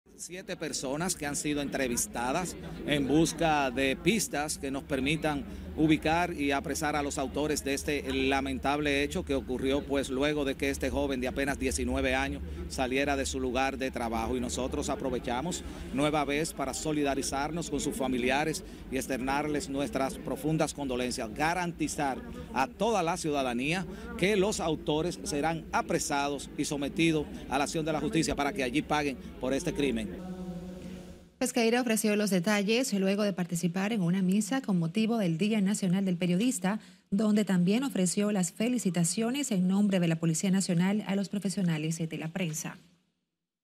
Policía interroga a varias personas por muerte de joven en Villa Mella
La Policía Nacional informó que avanza en las investigaciones para detener a los presuntos antisociales que causaron la muerte a un joven de 19 años durante un atraco en Villa Mella para despojarlo de su motor, luego de salir de su trabajo el pasado lunes.